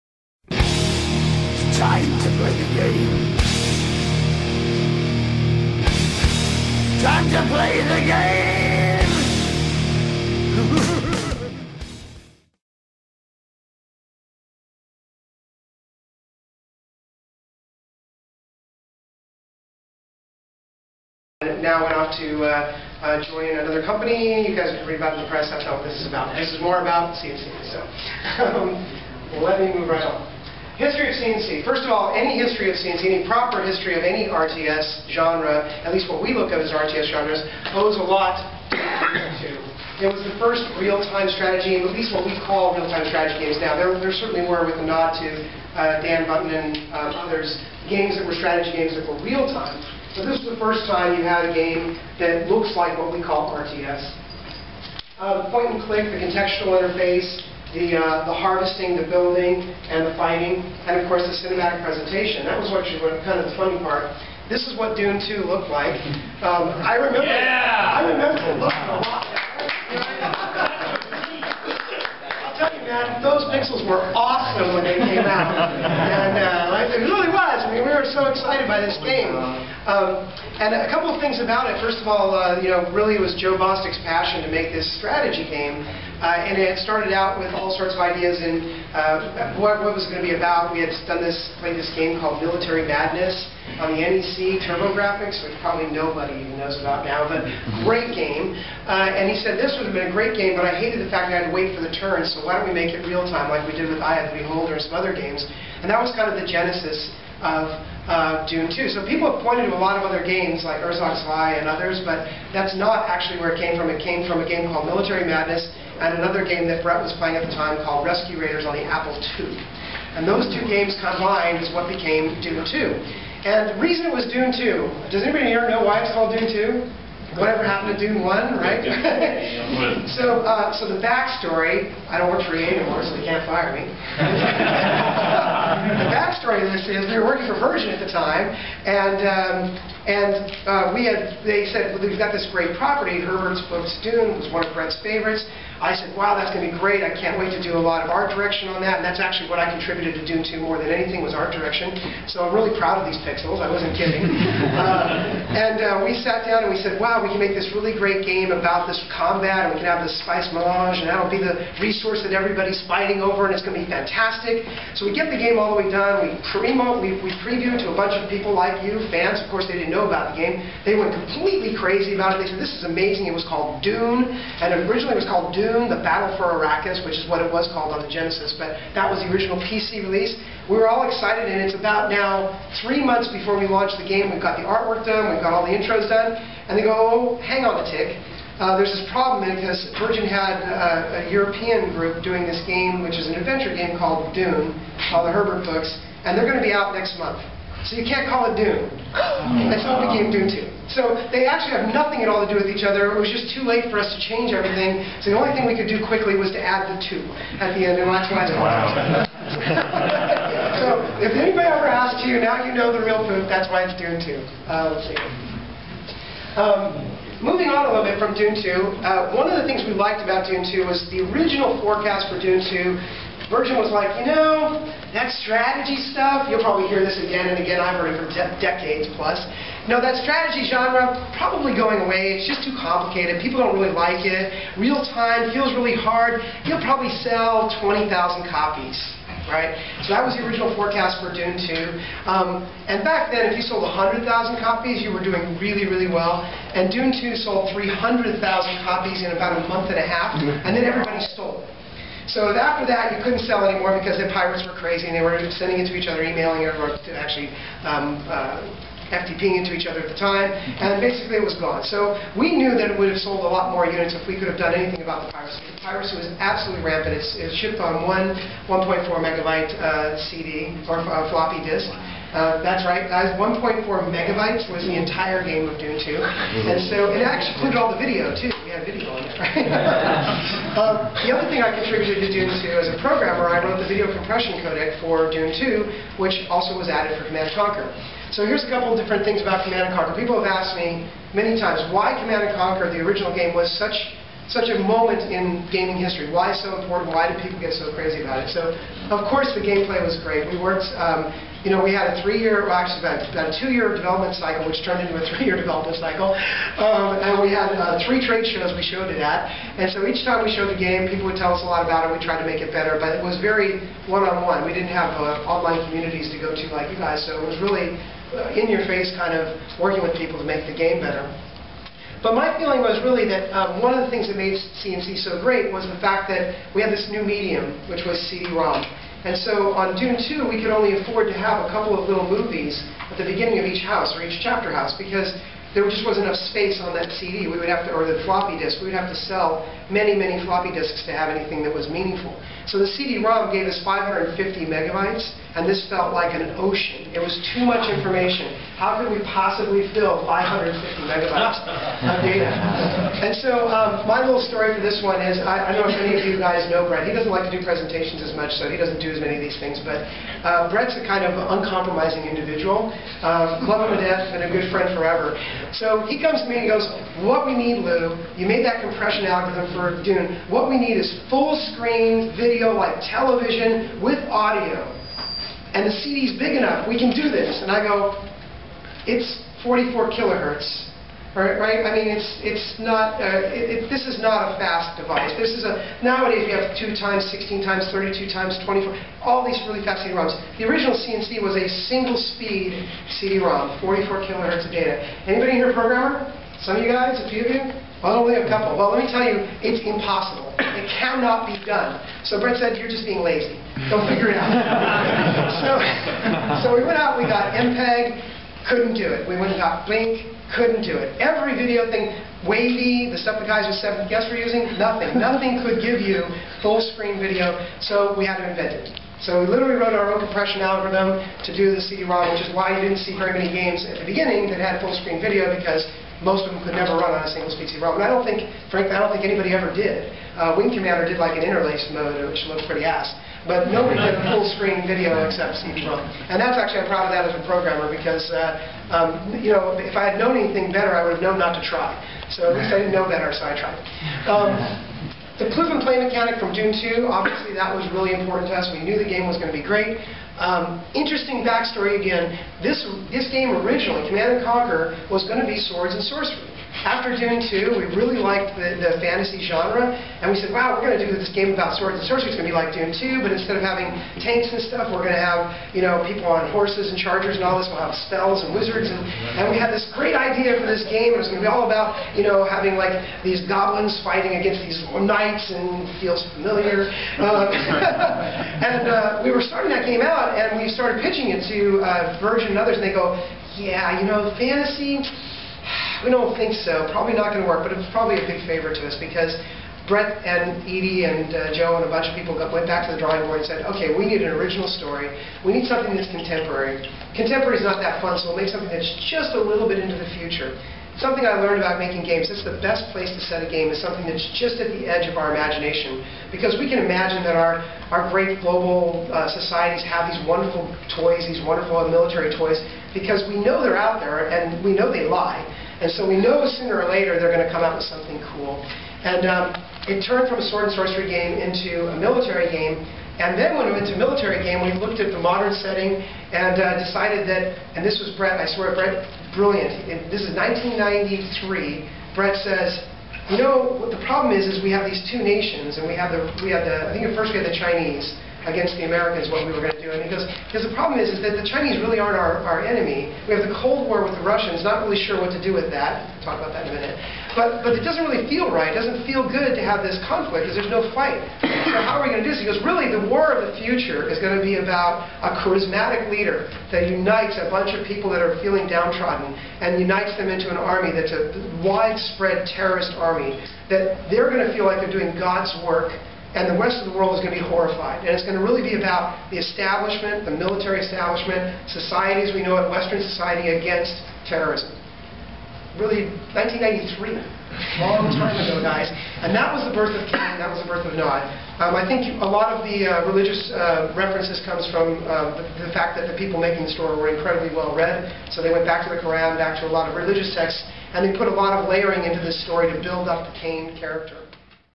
Louis Castle CommandCom Presentation - History of C&C Part 1
Here is a real special treat for all C&C fans. This is only presentation from CommandCom 2009 that we are allowed to show you. Relive the history of the Command & Conquer franchise with one the original creators, Louis Castle. All of the games beginning with the classic Command & Conquer, the Red Alert series and Generals are all covered. Louis even recalls funny and interesting facts from the years at Westwood and more. The video is in 3 parts.